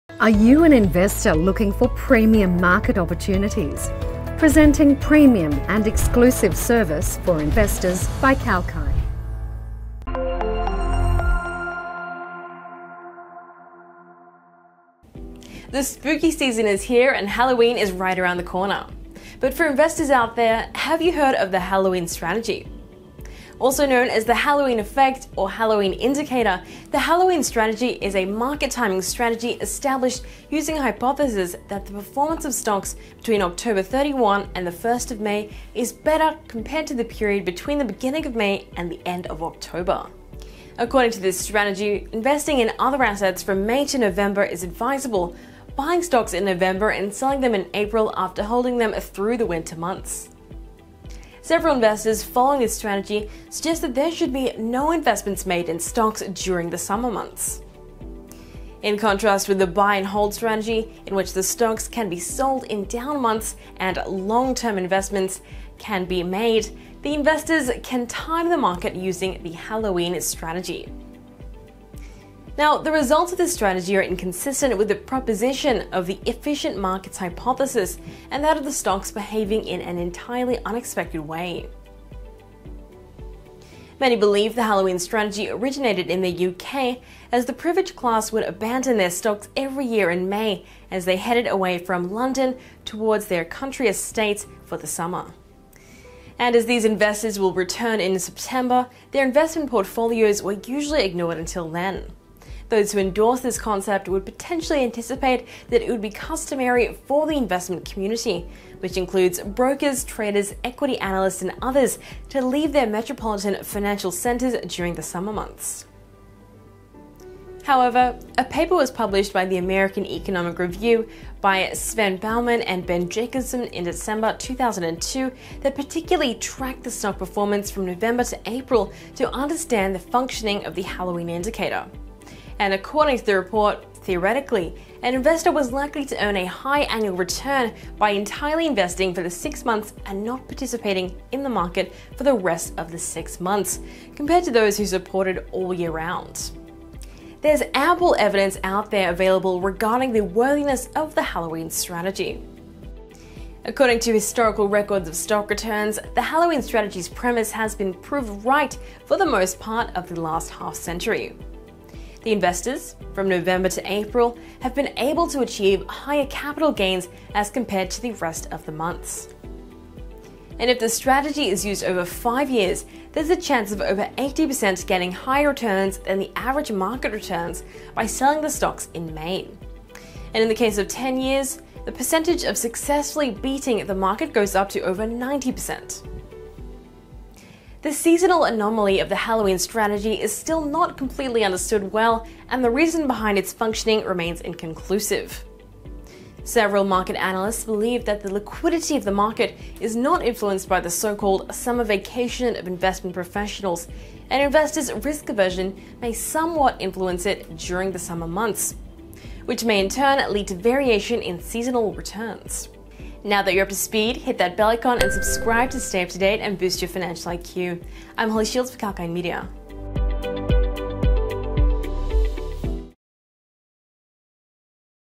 What is Halloween Strategy? | Kalkine Media
Halloween Strategy, also known as the Halloween effect or Halloween indicator, is a market-timing strategy established using a hypothesis that the performance of stocks between 31 October and 1 May is better compared to the period between the beginning of May and the end of October. According to this strategy, investing in other assets from May to November is advisable, buying stocks in November and selling them in April after holding them through the winter months. Several investors following this strategy suggest that there should be no investments made in stocks during the summer months.